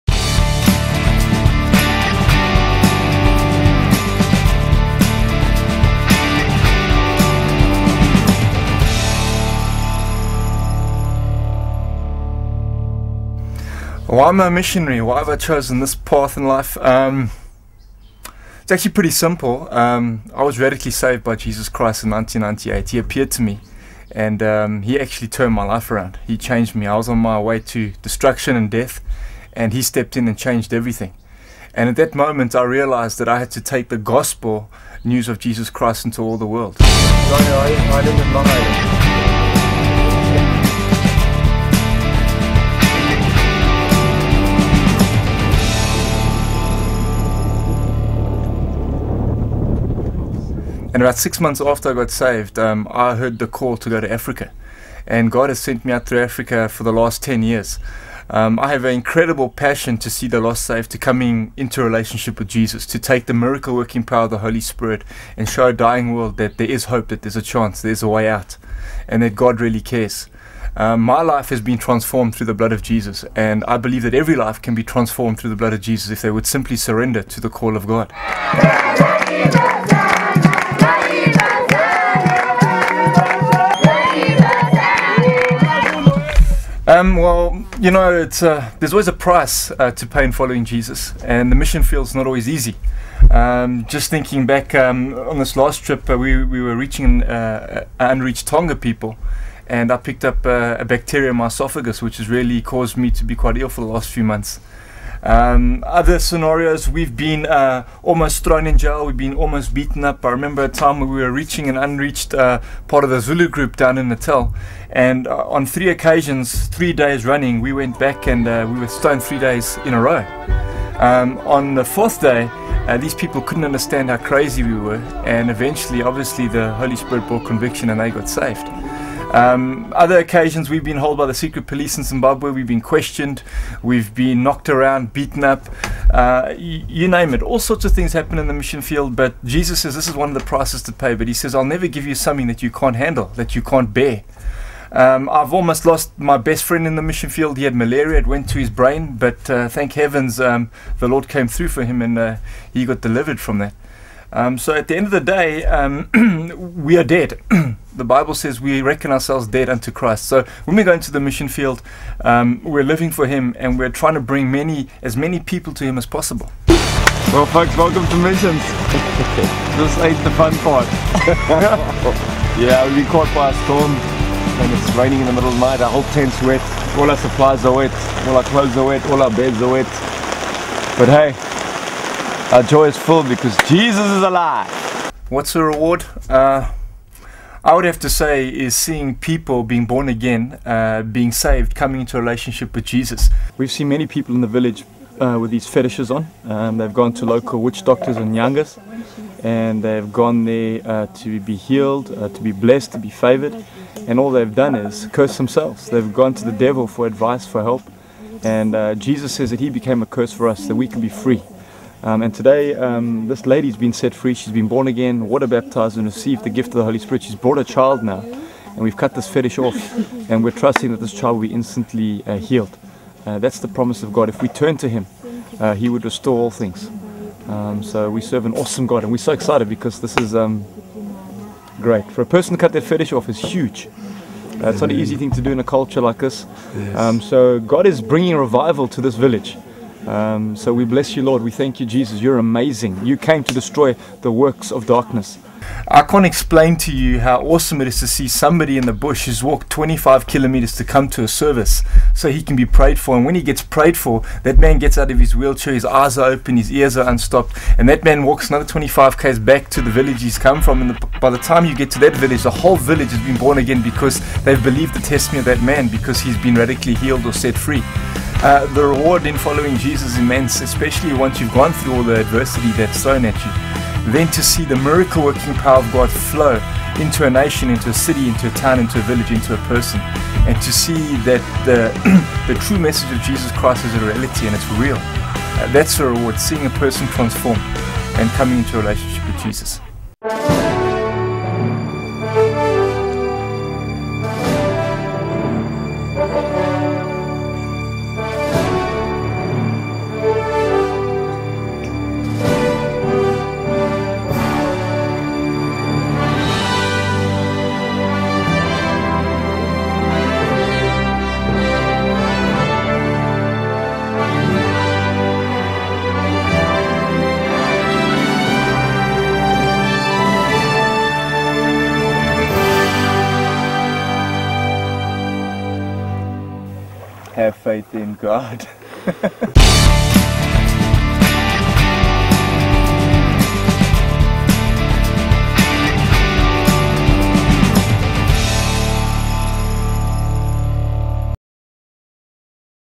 MMI Missions
This video is about taking the radical gospel of Jesus Christ to the tribes of Africa.